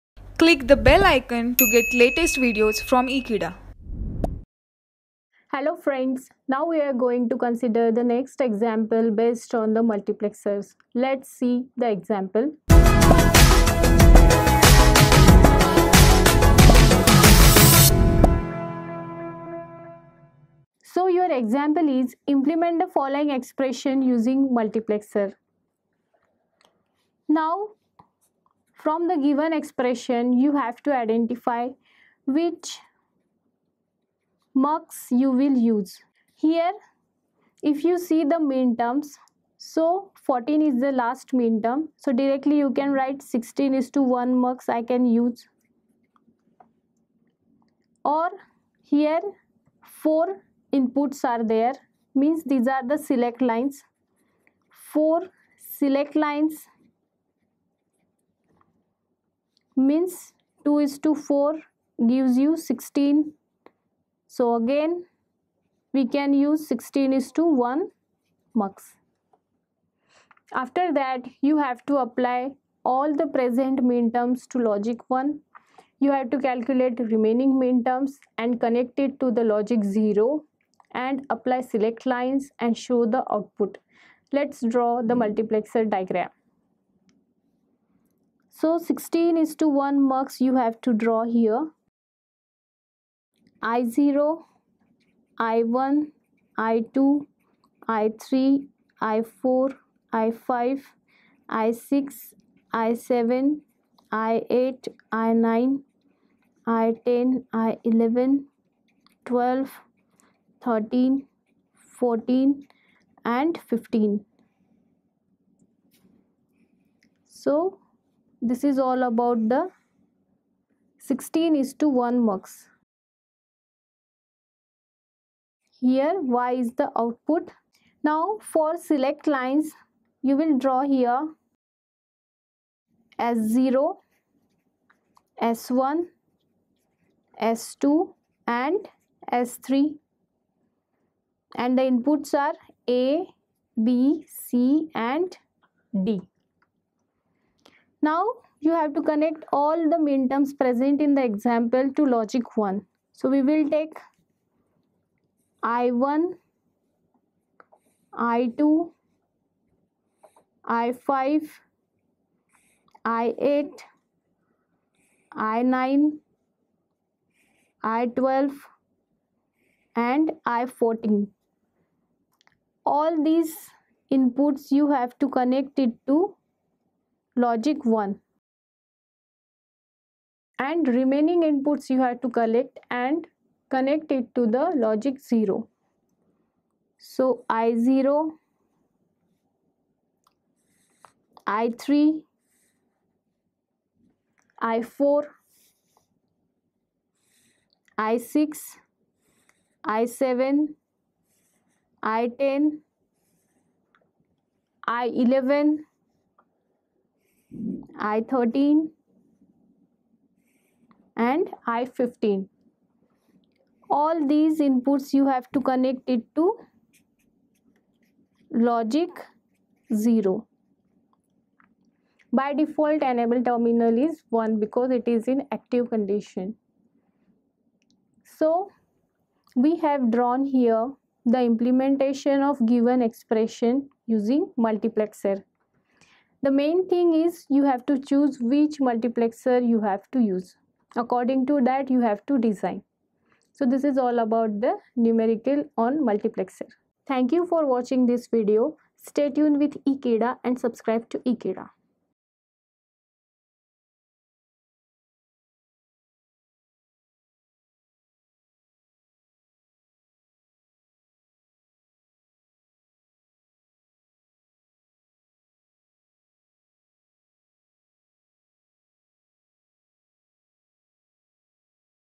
Numerical Based on Multiplexer Type 2 (Example 2) | Number System and Code | Digital Electronics
Explore the fundamentals of Digital Electronics with this insightful tutorial! Dive into 'Numerical Based on Multiplexer Type 2 (Example 2)' in the realm of Number System and Code. Unravel the intricacies of multiplexers and their application in digital systems through a practical example. Delve into the core concepts, solving numerical problems to master this essential topic. Whether you're a student or an enthusiast, join us to enhance your understanding and proficiency in Digital Electronics. Watch now for a comprehensive breakdown and clear explanations to ace your studies in this domain!

Access the Complete Playlist of Subject Digital Circuit Design -

Happy Learning.